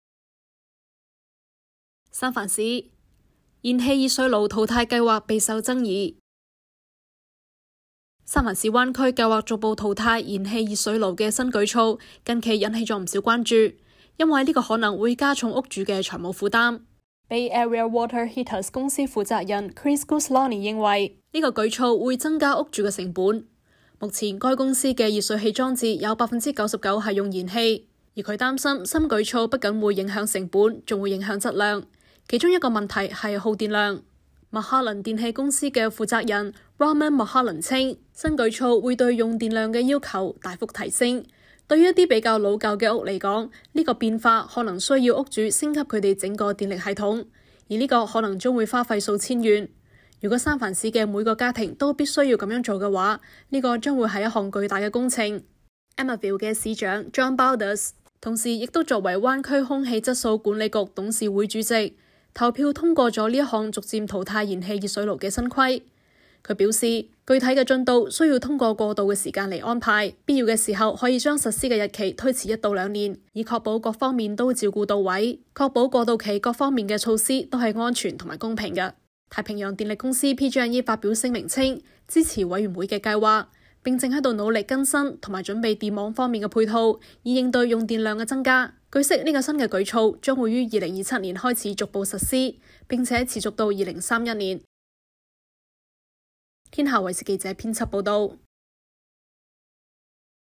【天下新聞】三藩市: 燃氣熱水爐淘汰計劃備受爭議 SF: Gas Water Heater Phase-Out Plan Sparks Debate
三藩市灣區計劃逐步淘汰
燃氣熱水爐的新舉措
近期受到了不少的關注
因為這可能加重屋主的財務負擔

Bay Area Water Heaters公司負責人Chris Guslani認為
這一舉措會增加屋主的成本
目前該公司的熱水器裝置中有99%是用燃氣
而Guslani擔心新舉措不僅會影響成本
還會影響質量
其中一個問題是耗電量
Mulholland電氣公司負責人Ronan Mulholland稱
新舉措對用電量的要求會大幅提升
對於一些比較老舊的房子來說
這一變化可能需要
房主升級他們的整個電力系統
這可能將花費數千元
如果三藩市的每個家庭都必須這樣做的話
那將是一項巨大的工程

Emeryville市長John Bauters
同時也作為灣區空氣質素管理區董事會主席
投票通過了這項逐步淘汰燃氣熱水爐的新規
Bauters表示具體進度
需要根據過渡時間來安排
必要的時候可以把實施日期推遲一兩年
以確保所有方面都照顧到位
確保過渡期是各方面措施是安全和公平的

太平洋電力公司PG&E發表聲明稱
支持委員會的計劃
並正在努力更新和準備電網方面的配套
以應對用電量的增加
據悉這個新的舉措
將於2027年開始逐步實施
並持續到2031年